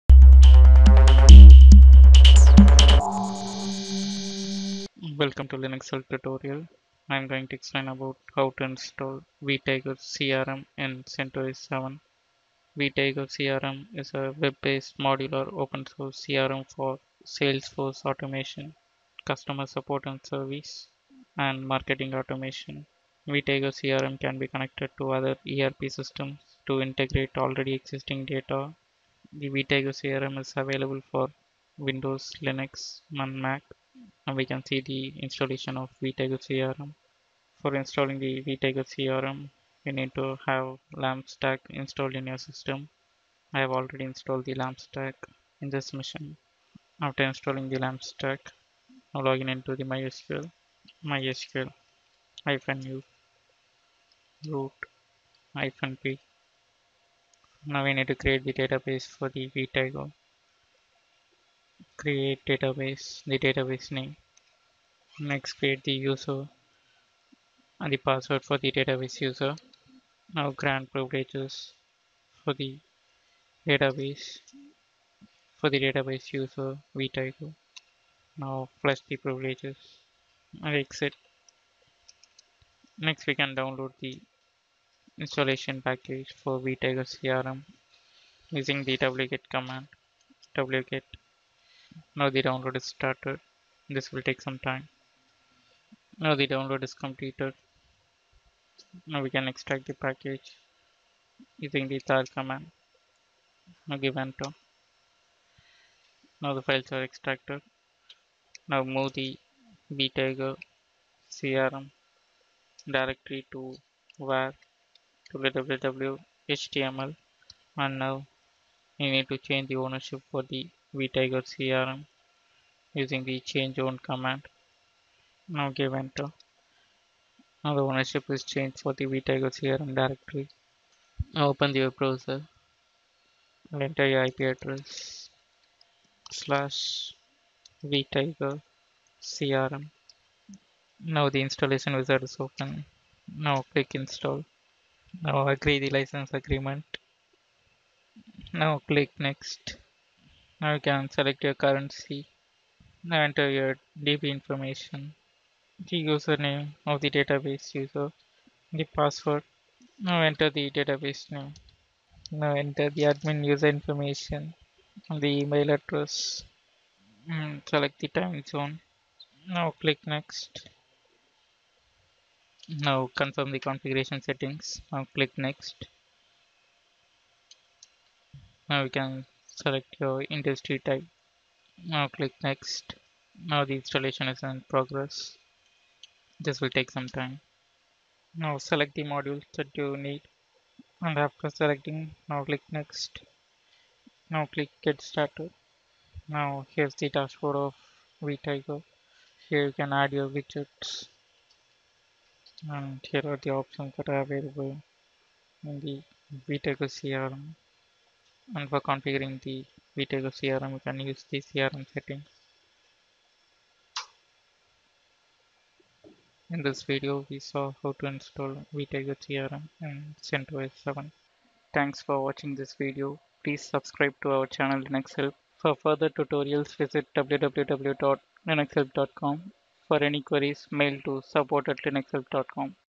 How to install vTiger CRM in CentOS 7
This video covers the installation procedure vTiger CRM in CentOS 7. vTiger CRM is a cross-platform, web based, modular, open-source CRM for various customer integration purposes.